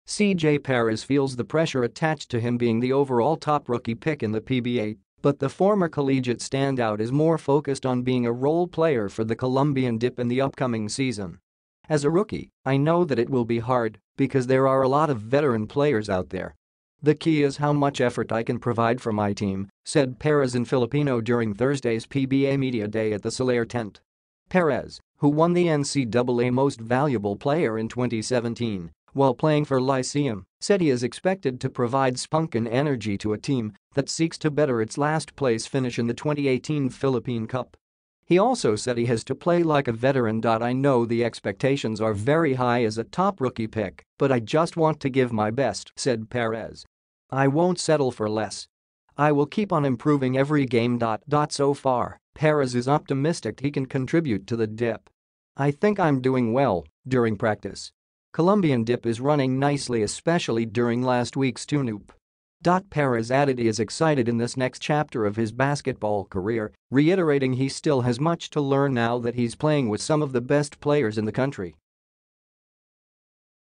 Perez feels the pressure of a No. 1 draft pick
CJ Perez feels the pressure attached to him being the overall top rookie pick in the PBA, but the former collegiate standout is more focused on being a role player for the Columbian Dyip in the upcoming season.

“As a rookie, I know that it will be hard because there are a lot of veteran players out there. The key is how much effort I can provide for my team,” said Perez in Filipino during Thursday’s PBA Media Day at the Solaire Tent.

Perez, who won the NCAA Most Valuable Player in 2017 while playing for Lyceum, said he is expected to provide spunk and energy to a team that seeks to better its last place finish in the 2018 Philippine Cup.

He also said he has to play like a veteran.

“I know the expectations are very high as a top rookie pick, but I just want to give my best,” said Perez. “I won’t settle for less. I will keep on improving every game.”

So far, Perez is optimistic he can contribute to the Dyip.

“I think I’m doing well (during practice). Columbian Dyip is running nicely especially during last week’s tuneup.”

Perez added he is excited in this next chapter of his basketball career, reiterating he still has much to learn now that he’s playing with some of the best players in the country.